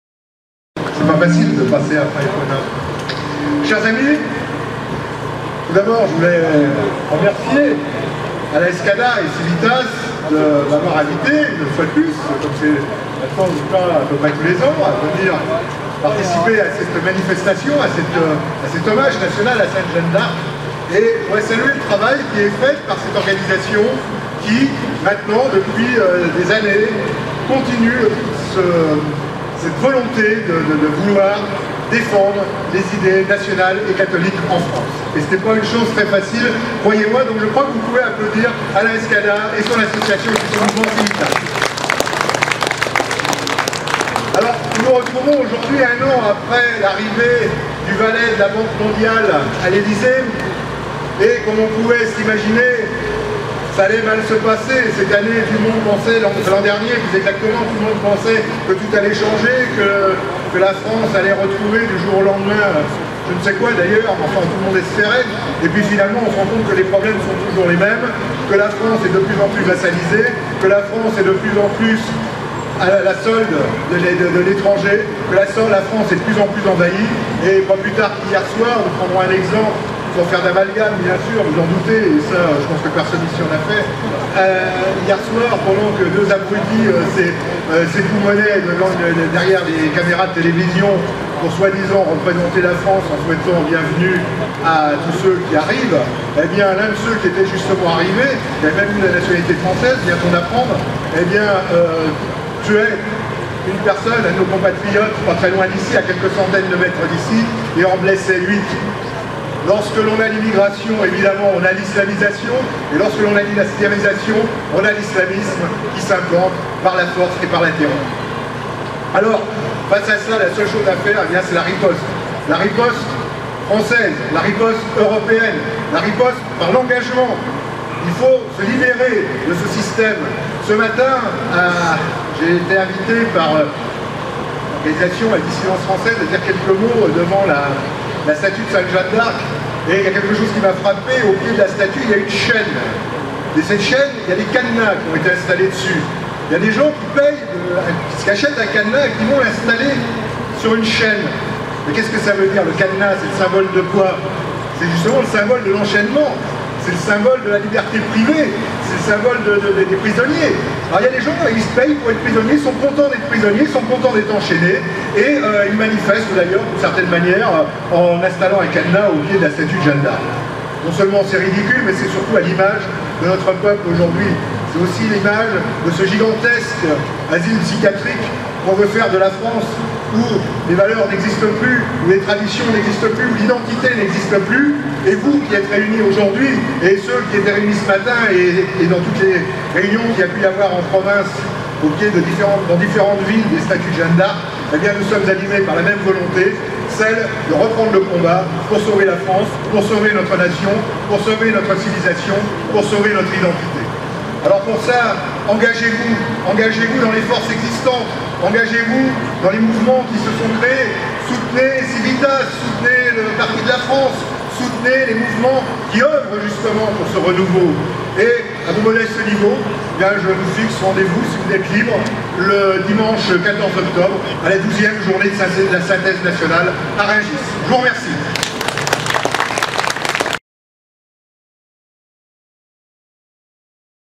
"L'engagement pour riposte face à l'immigration" Roland Hélie
Retrouvez le discours de Roland Hélie, Directeur de Synthèse Nationale, lors du défilé du 13 mai 2018 à Paris organisé par Civitas en l'honneur de Sainte Jeanne d'Arc.